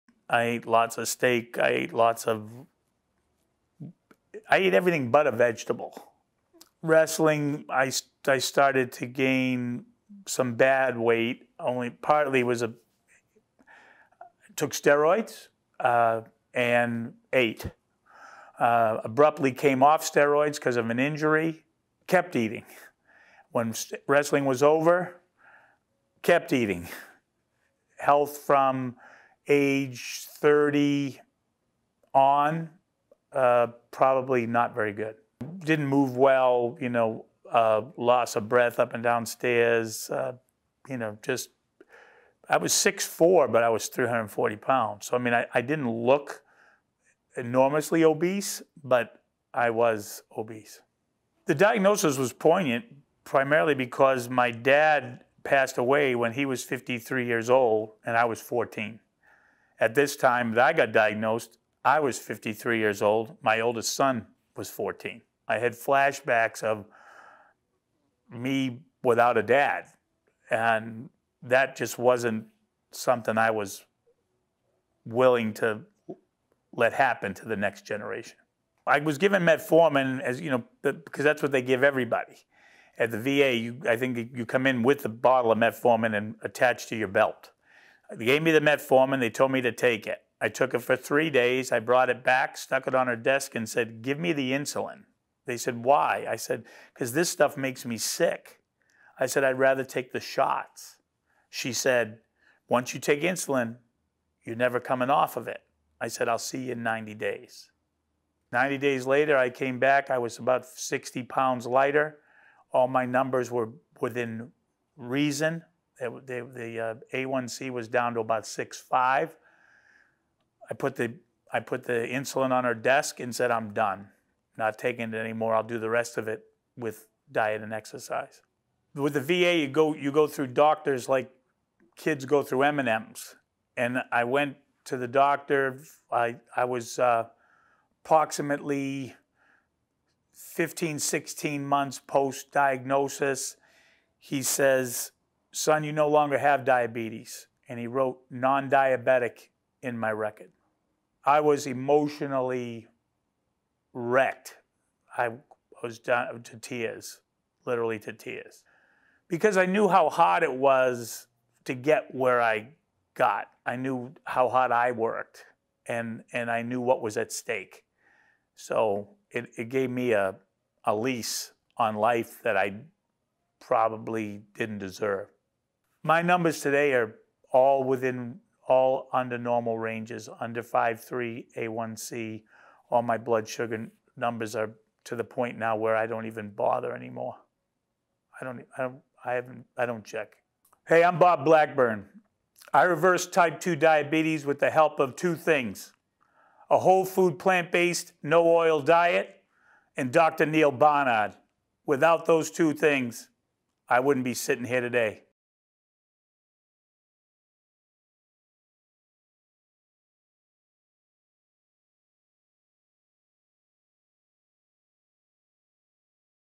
Reversing Diabetes with a Plant-Based Diet | Bob's Story
Bob Blackburn didn't want to leave his son without a father. When he was diagnosed with diabetes, he was told he'd never get off of insulin. He changed his diet, and today his blood sugar levels are completely in the normal range!

Read Bob's full story and learn more about how foods affect our hormones in Dr. Barnard's new book, Your Body in Balance: The New Science of Food, Hormones, and Health, which debuts on Feb. 4, 2020.